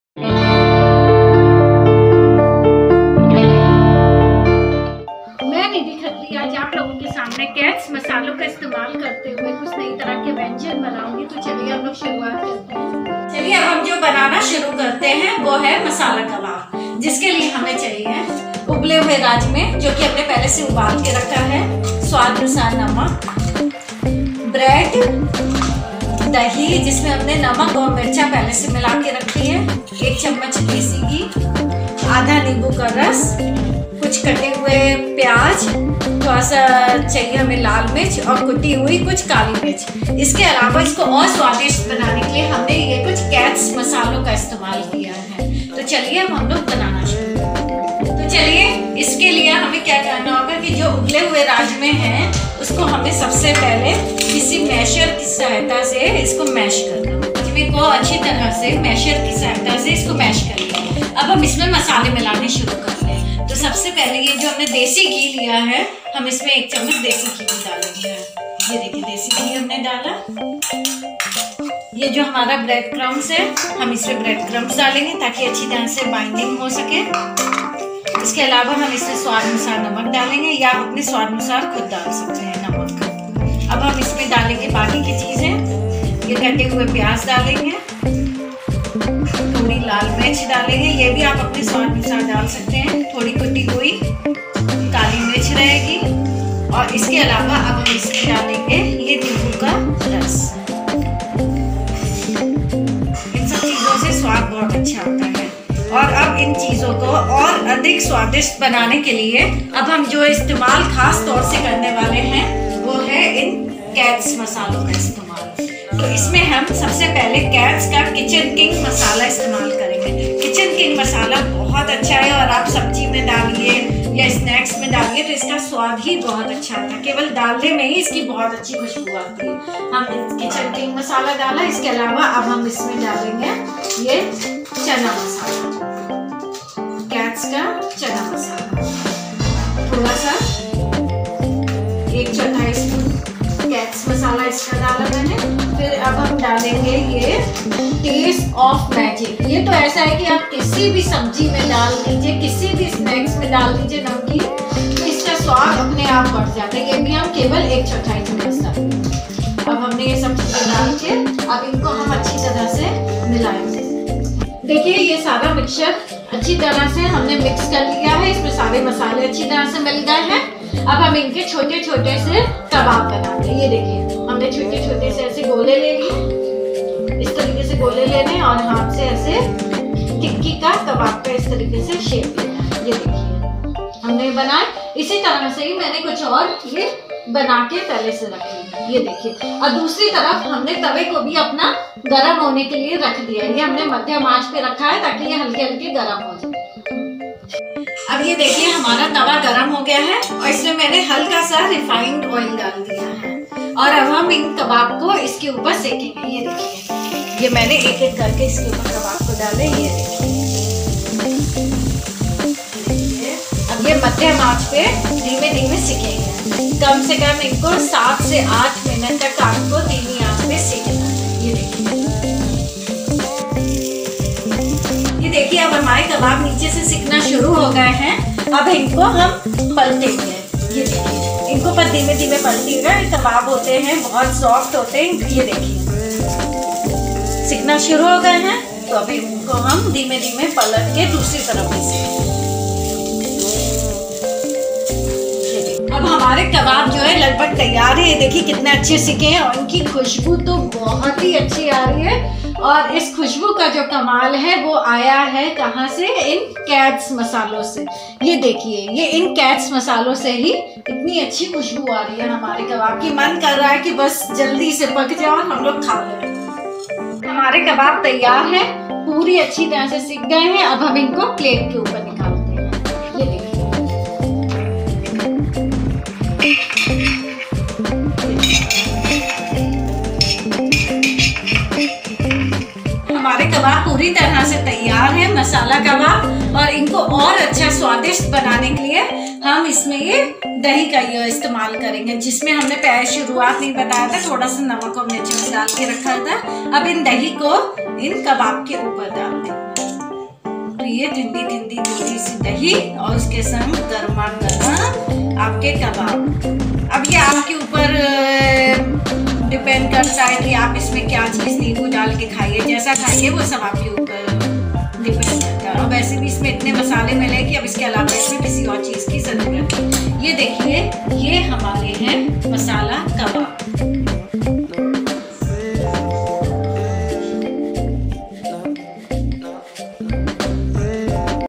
Masala Kabab Nidhi Khattry || Cates spices || Chef in You || Lucknow UP || Events Just abhi
Contestant - NIDHI KHATTRY
RECIPE - Masala Kabab
CATES - CHEF IN YOU (Maa Ke Hathon Ka Jaadu Ab Cates Ke Masalo Main)